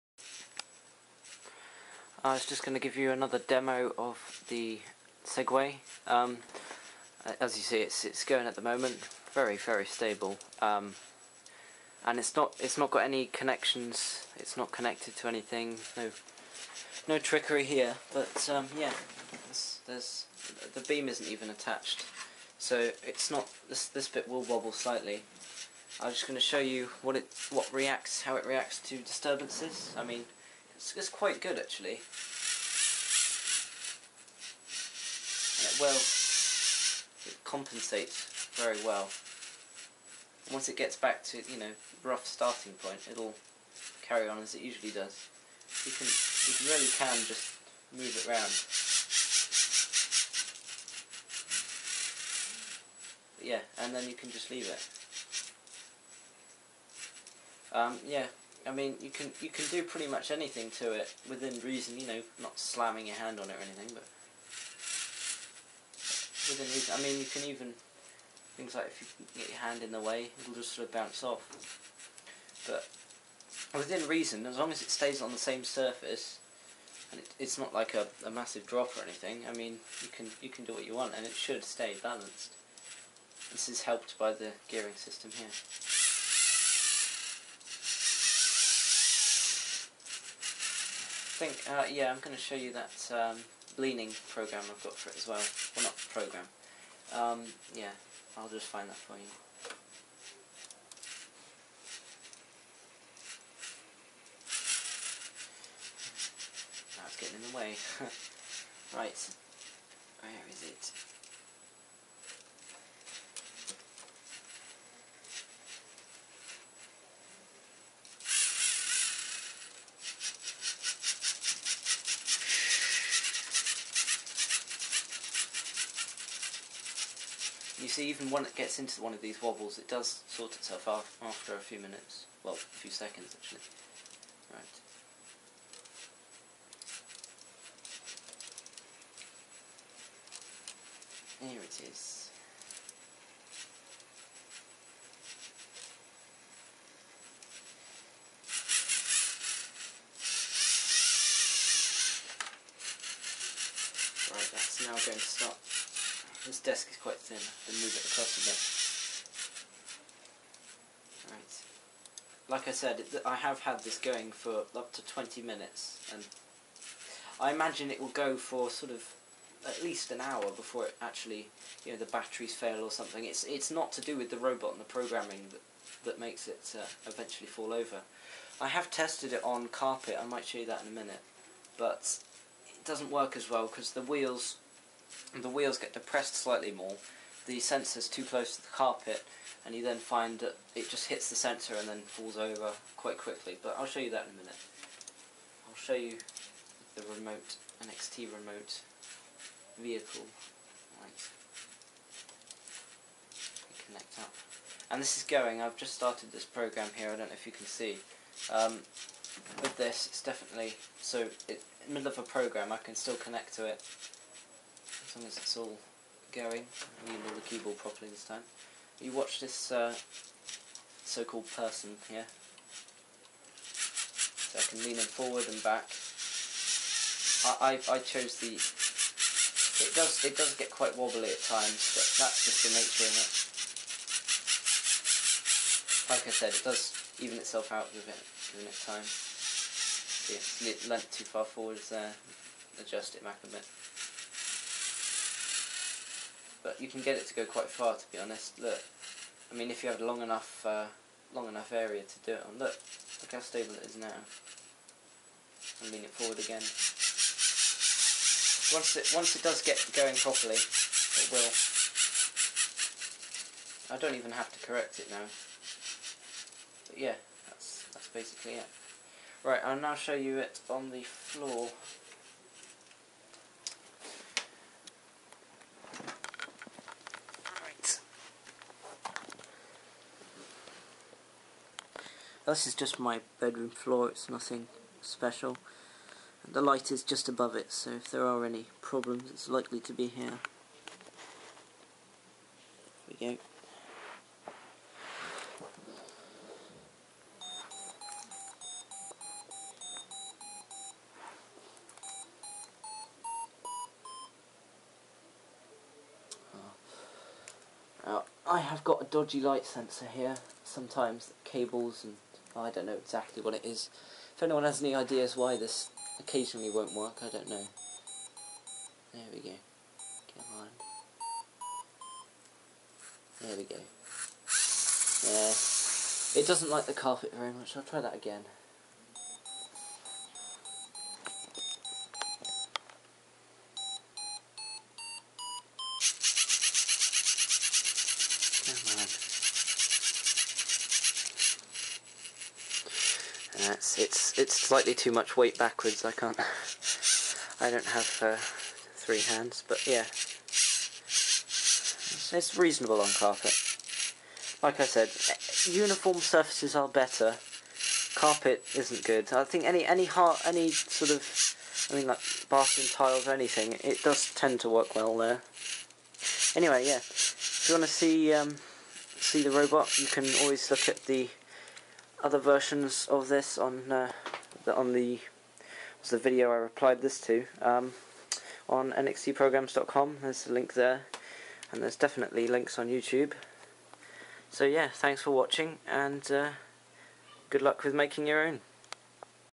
Nxt Segway using light sensor (very stable!) (Part 2 Robot demo)
This is a modified version of the 'NXT Segway with rider' on nxtprograms.com, with help from it's designer I have improved the stability when balancing, and modified different parts of the robot and program to allow the best possible result.

Below is the program I modified, in the program I only added an averaging loop for the centre value, so read the value and record it 5 times and divide by 5, which gives a better average to prevent drift, all the instructions are on nxtprograms.com.